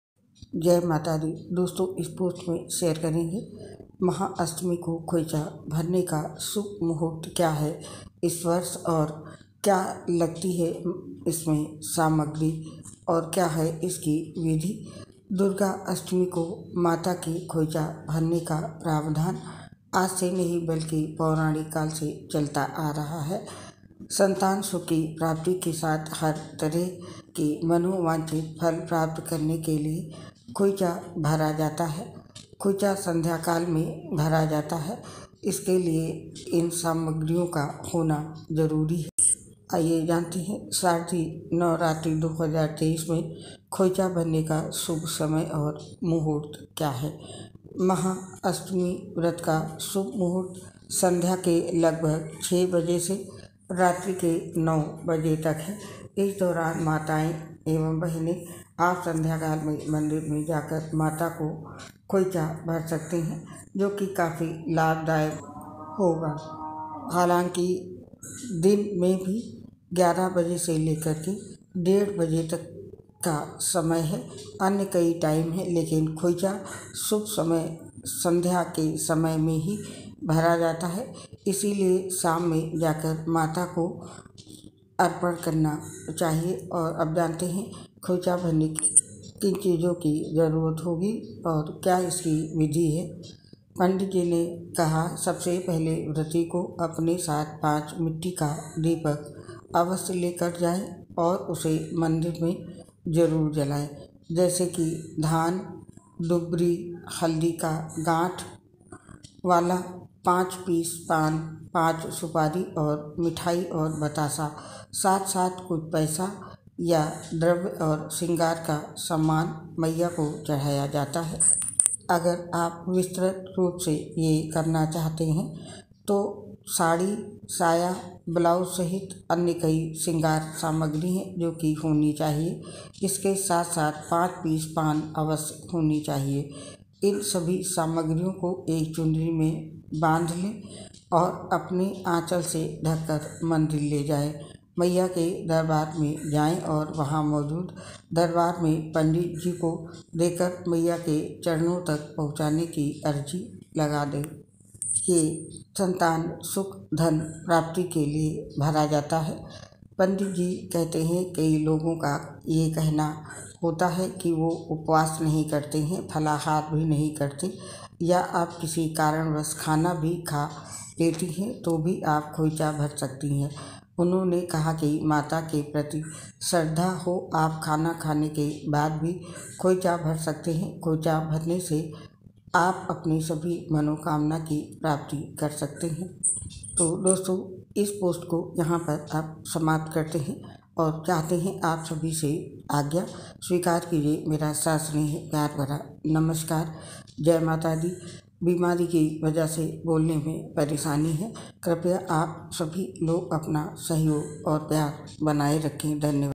शारदीय नवरात्रि 2023 खोइचा भरने का शुभ समय मुहूर्त खो॓इचा भरने की सम्पूर्ण विधि जानिए माता को खोइचा
ये जानकारी धार्मिक ज्योतिष शास्त्र पंचाग पर आधारित है
शारदीय नवरात्रि 2023 में माता का ,खोइचा भरने का शुभ समय मुहूर्त और
खोइचा भरने की सम्पूर्ण विधि